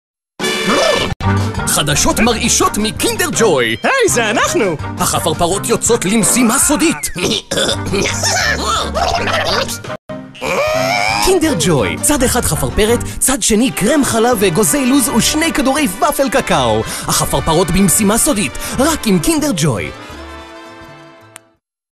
Hebrew male voiceover sample - Zvika F.
Hebrew male voiceover talent Zvika F. is ready to voice your next upcoming video's Hebrew version in Israel:
Zvika F. is also an English voiceover talent.